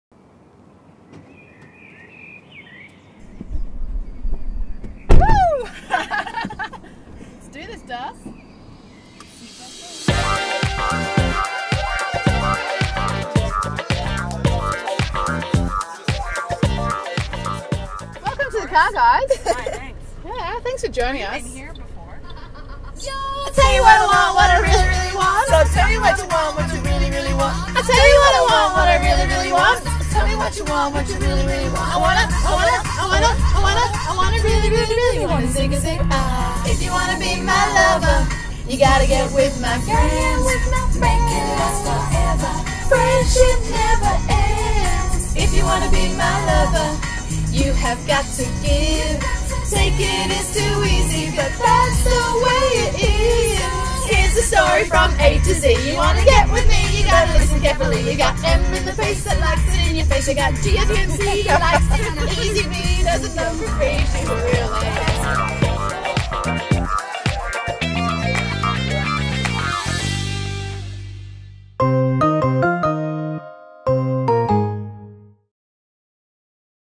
Mike Carney Carpool Karaoke Ep.8
Carpool Karaoke continues this week thanks to Mike Carney Toyota.

Check out championship heroes Darcee Garbin and Rachael McCully – along with two special Fire guests - as they celebrate retro round!